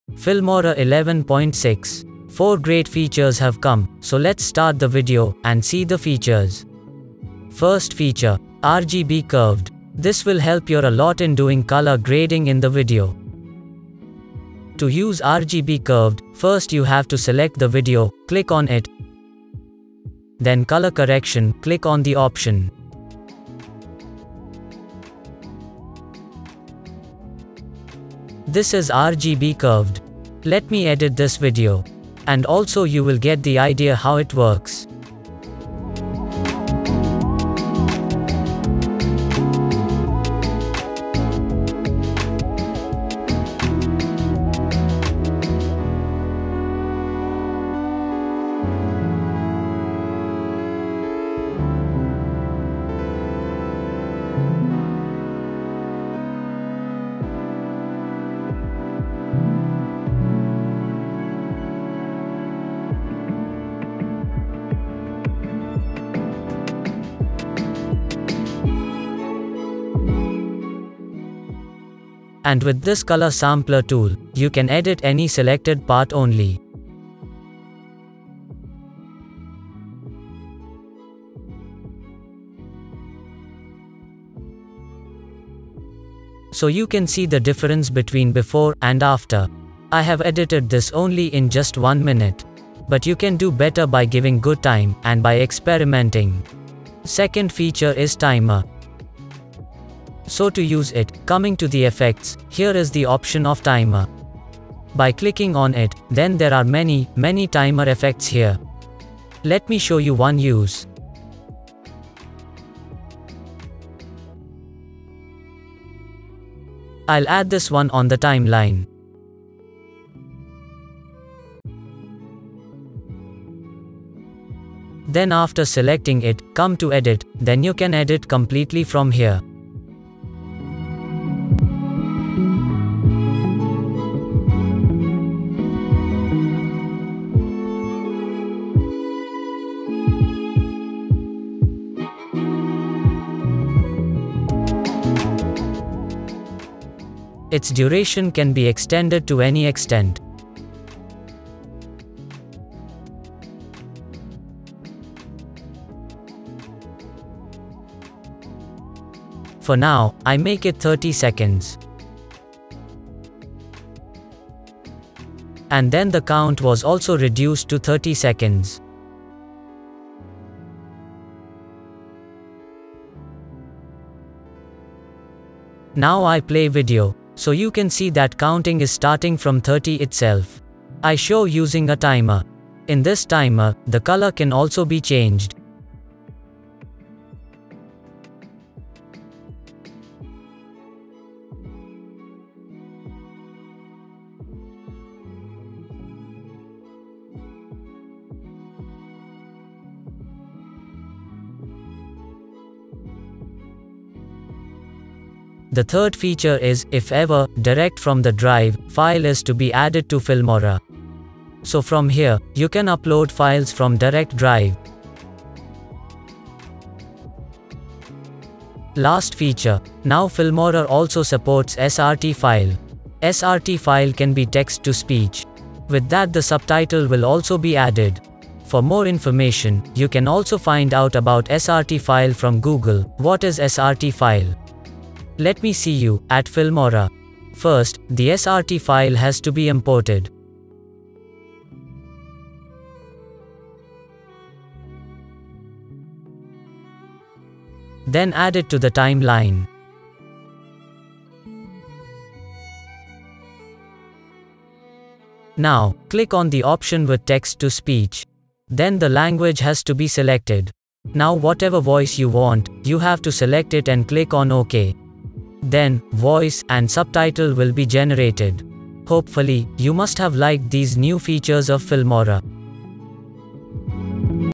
Filmora New Update Version 11.6 | Filmora New Features
New Wondershare  Filmora Cool Features Tools | New Filmora Update

Filmora Lower Third Part - 1

Custom Design -
Do you Need a Thumbnail, YouTube Intro, Banner, Outro, Live stream overlay, Lower third animation, and more? YOU ARE AT THE RIGHT PLACE.

Spidy Tuts